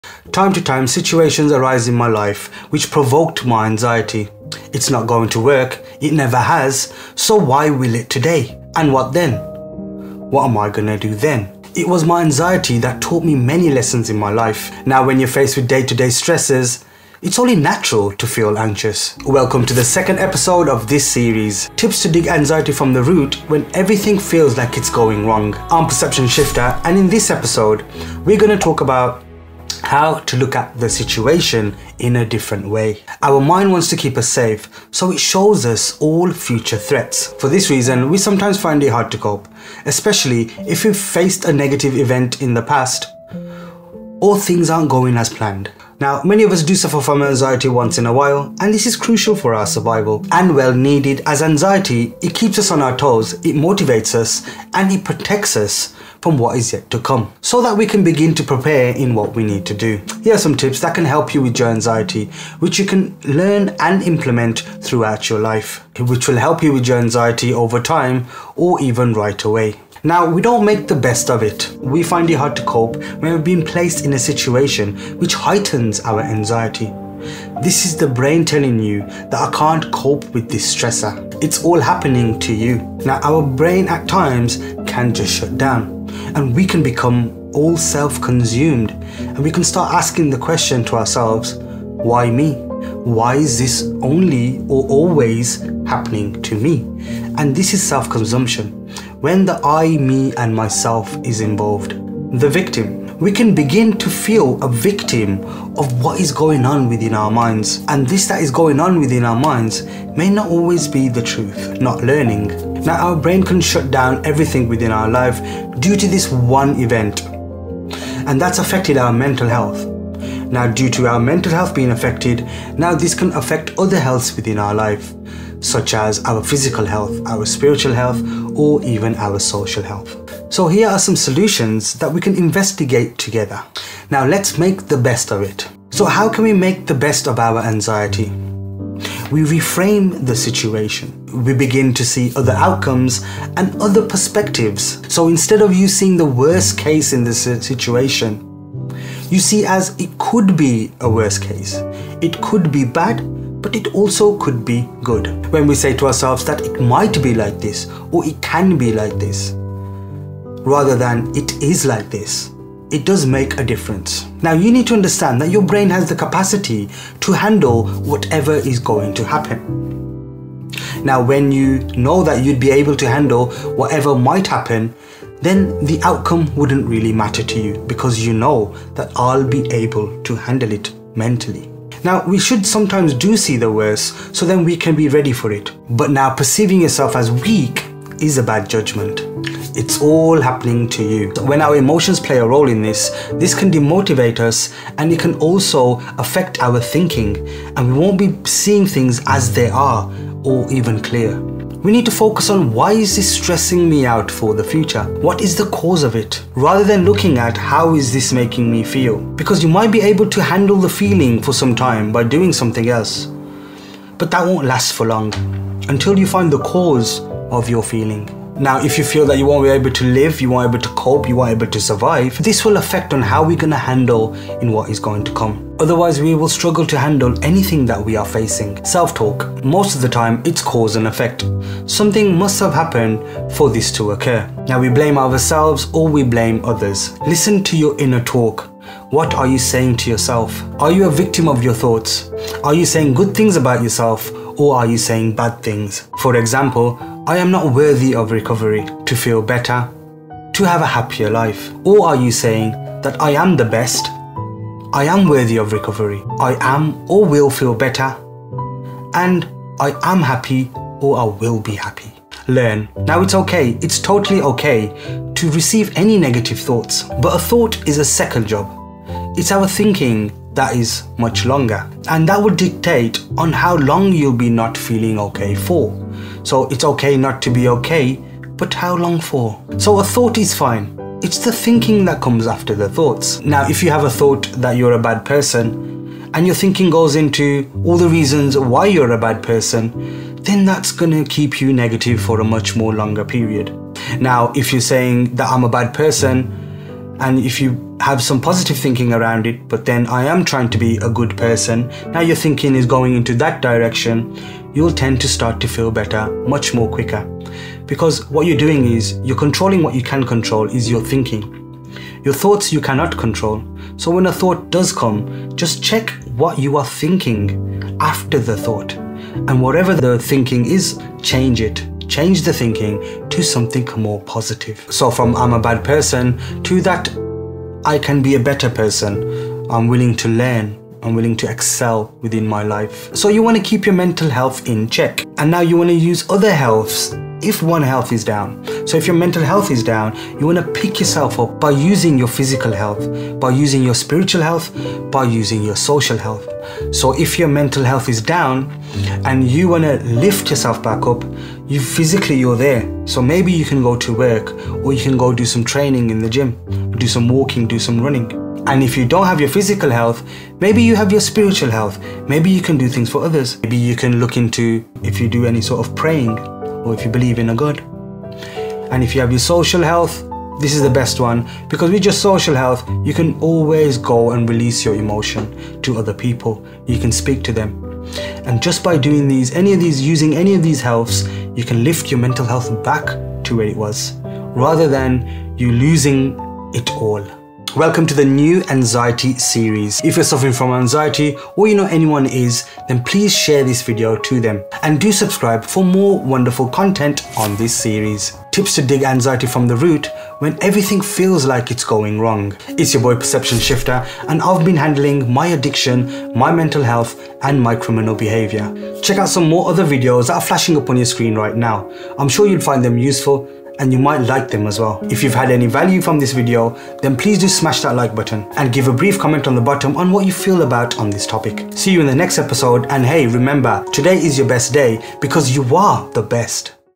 Unlocking Perspective: Coping with Anxiety - How to Shift Your Perception of Situations E2
In this insightful video, we dive into the realm of anxiety and explore the common challenge of viewing situations from a negative or distorted perspective. Join us as we unravel the importance of shifting our perception and looking at situations in a different way. Discover practical solutions and valuable tips to break free from negative thinking patterns and gain a more empowering outlook. This is the second tip in our series dedicated to coping with anxiety, providing you with actionable strategies to address anxiety head-on, whether immediately or over time. Get ready to unlock a new perspective and embrace a path towards greater clarity and peace of mind. Don't miss out on this enlightening video! Remember, today is your best day! 💙🙏